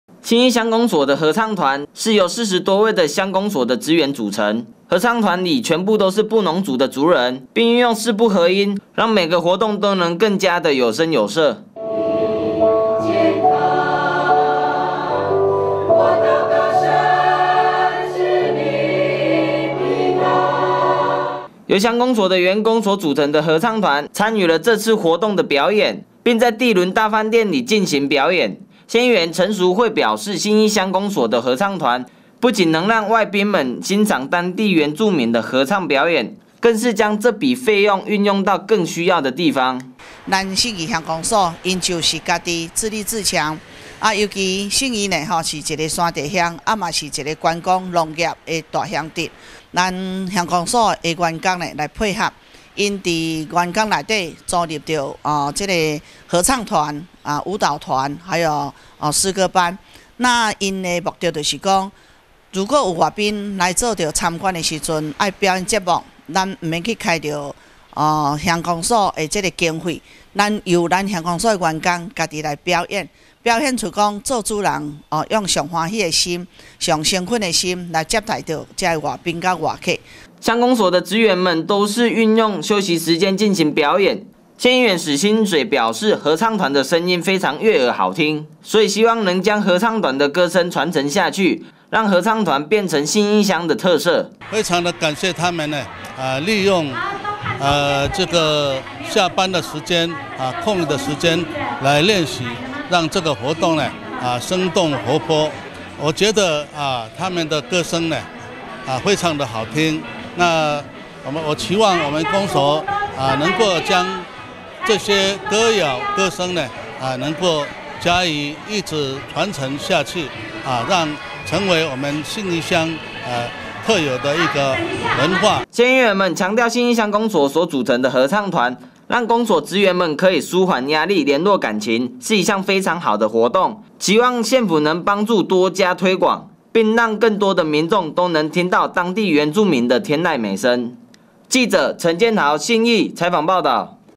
20170816 民議新聞 信義鄉合唱團 傳承原鄉文化原音再現(縣議員 史清水、陳淑惠)
由信義鄉公所組成的布農族合唱團成員都是鄉公所的職員運用4部合音進行合唱，而縣議員史清水與陳淑惠表示鄉公所舉辦此次活動不僅能推廣布農文化更將創新的理念帶入這次的活動中，讓每次活動多了一份特色使原住民的天籟美聲能讓世界聽到。